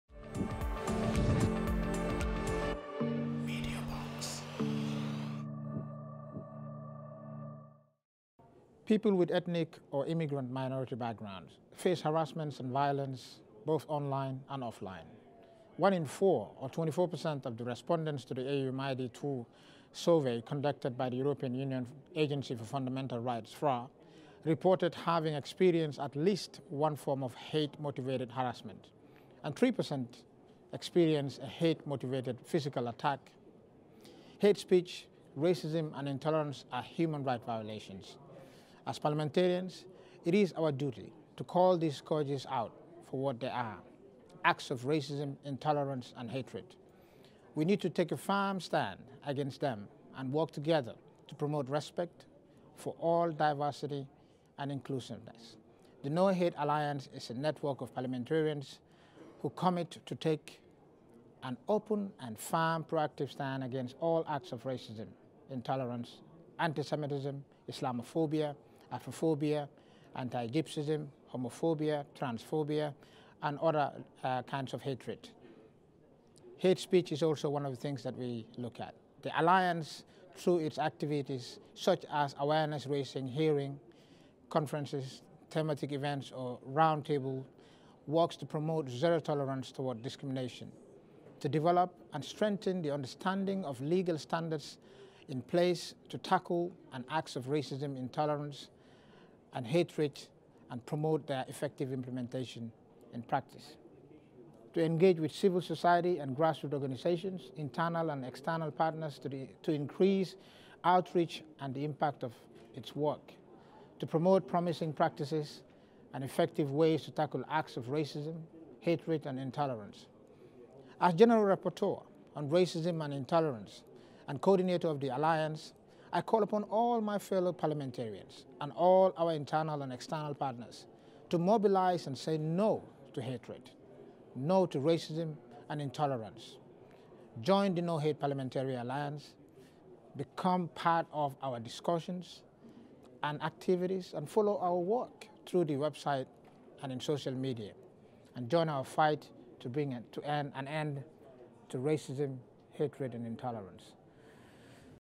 Momodou Malcolm Jallow - No Hate Parliamentary Alliance
Mr Momodou Malcolm Jallow, General Rapporteur on combating racism and intolerance and Coordinator of the No Hate Parliamentary Alliance, describes the priorities of his term as Coordinator and the planned activities of the No Hate Parliamentary Alliance.